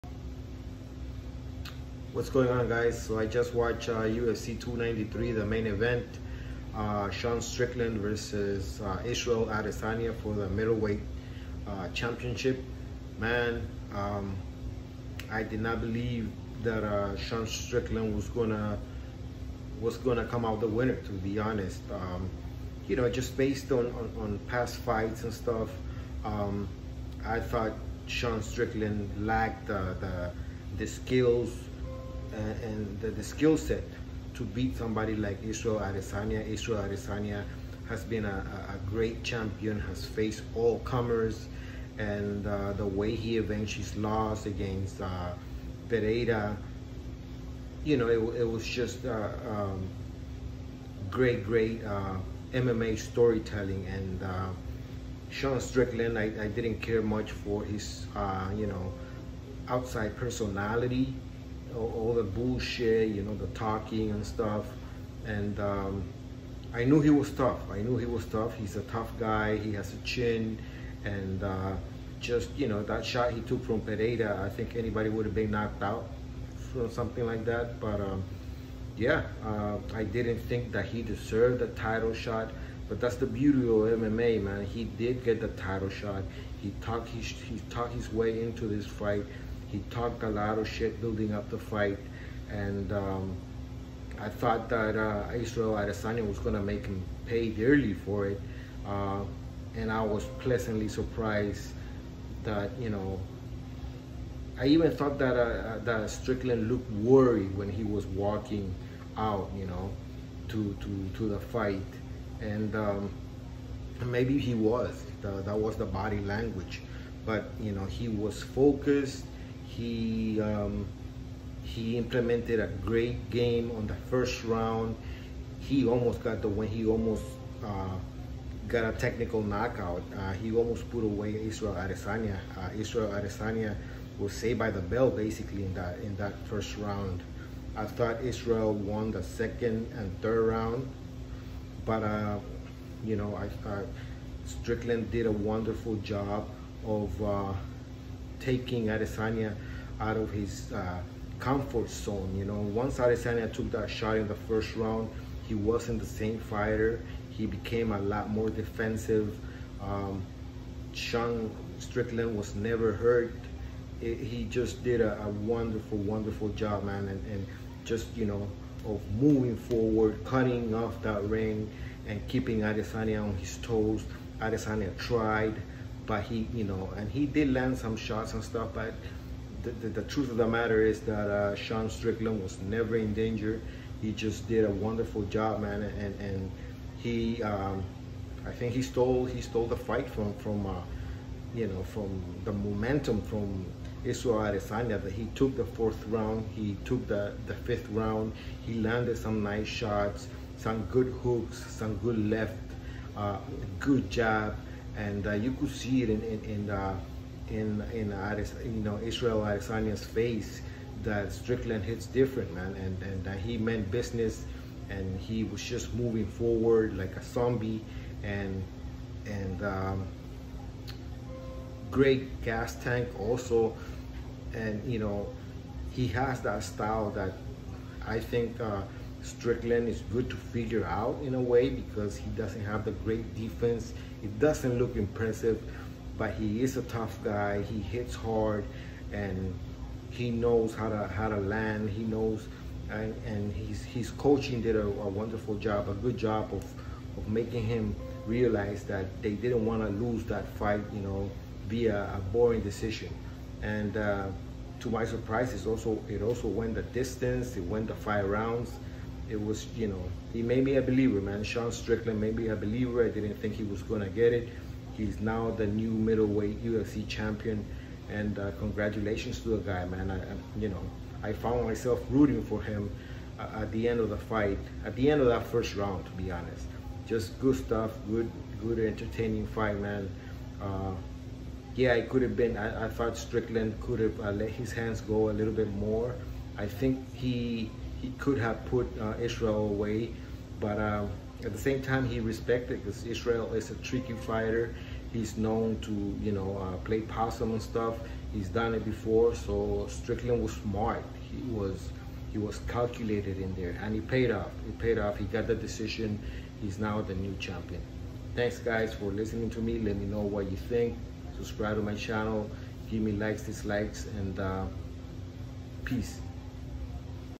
UFC 293 Israel Adesanya vs Sean Strickland: reaction and thoughts
And NEW UFC middleweight champion Sean Strickland!!! 😳